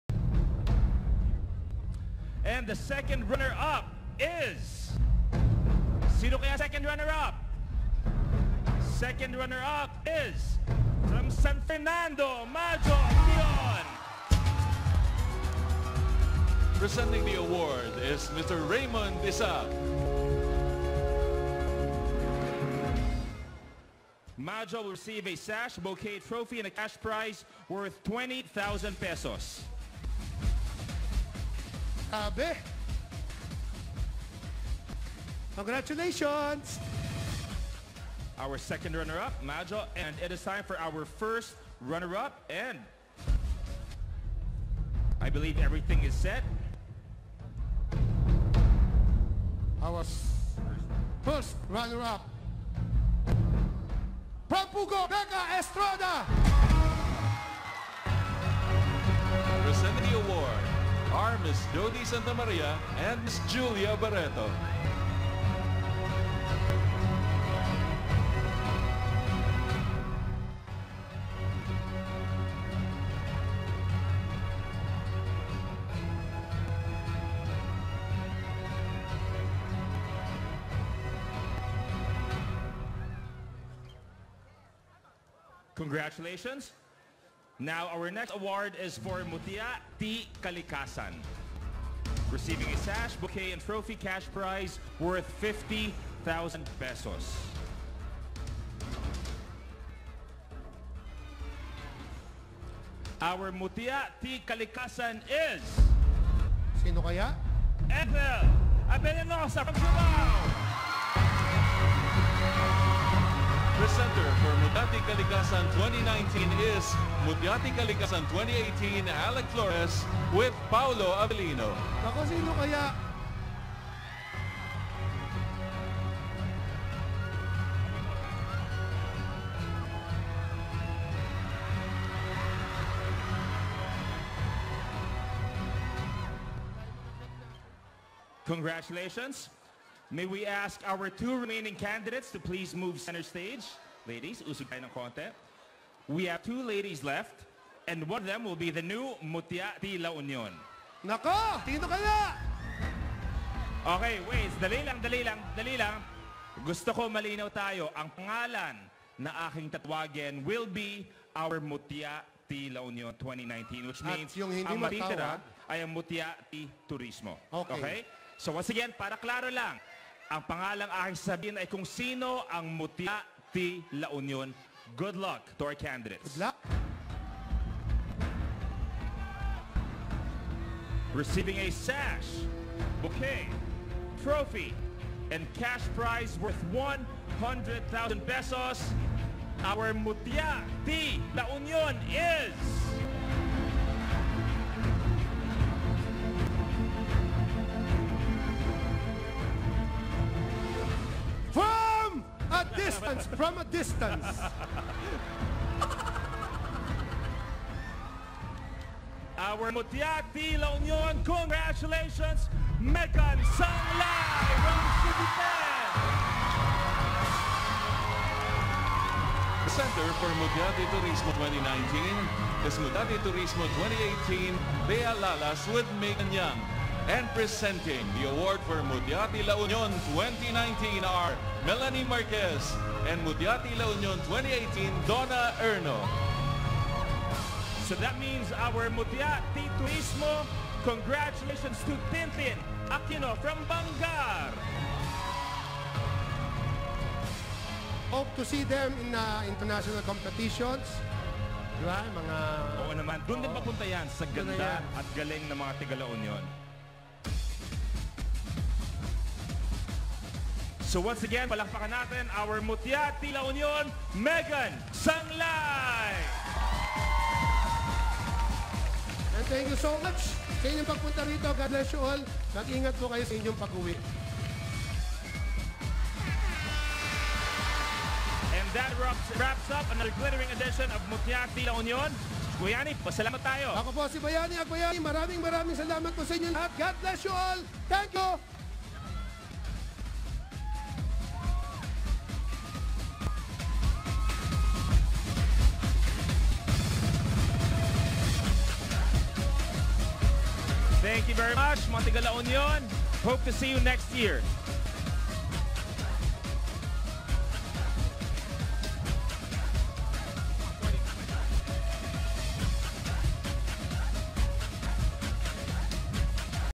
Winner and Crowning Moment MUTIA TI LA UNION 2019 GRAND CORONATION NIGHT
Mutia Ti La Union 2019 Meghan Sanglay

169th MUTIA TI LA UNION GRAND CORONATION NIGHT